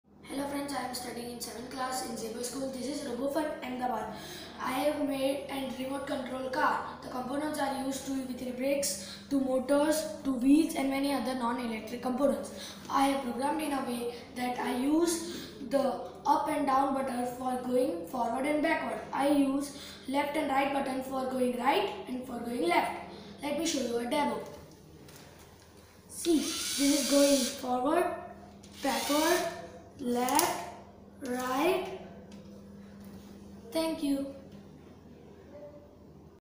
Lego Remote Control Car - Vansh Jobanputra - RFL Ahmedabad(8980172306)
Lego Remote Control  Car using EV3 Mindstorm kit. Using Bluetooth communication with two ev3 brick it will work. one EV3 brick is master so it will give the command and the other is slave so that EV3 brick will follow the command. That's how the RC Car will work. It constructed & programmed By : Vansh Jobanputra